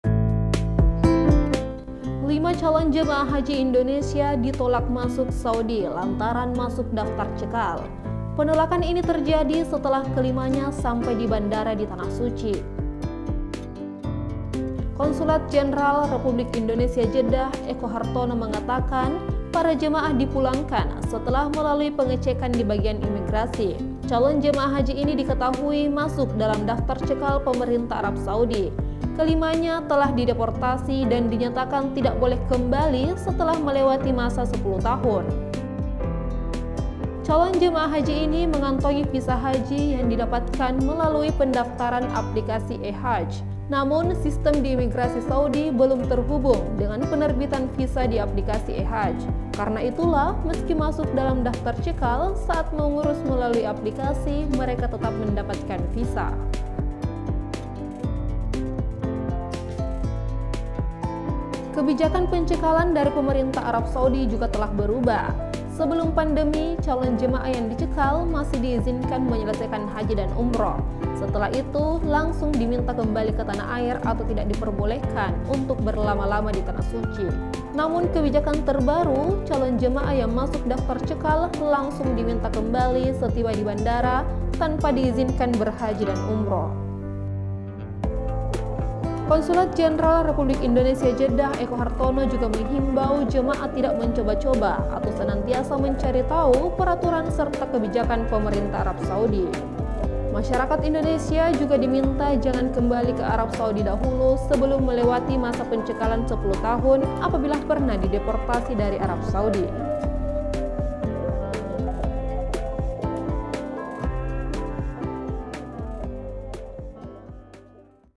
5 Calon Haji Asal Indonesia Dicekal Masuk Arab Saudi
Lima calon jemaah haji Indonesia ditolak masuk Saudi lantaran masuk daftar cekal. Para jemaah dipulangkan setelah melalui pengecekan di bagian imigrasi. calon jemaah haji ini diketahui masuk dalam daftar cekal pemerintah arab saudi. kelimanya telah dideportasi dan dinyatakan tidak boleh kembali sebelum melewati masa 10 tahun.